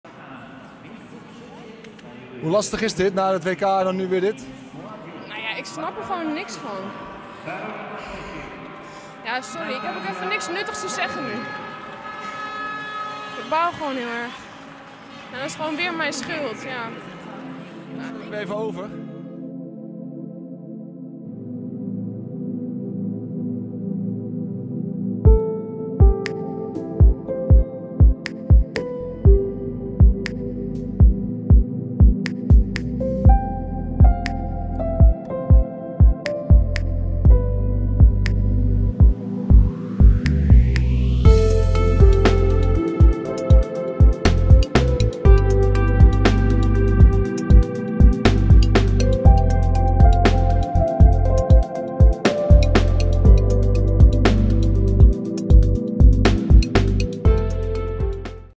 Joy Beune - World Cup Speed Skating (Poland 2023)
The Dutch skaters once again clumsily missed out on a gold medal in the team pursuit at the World Cup in Poland on Saturday (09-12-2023). Joy Beune forgot to wear her transponder and so the Netherlands' winning time was cut short.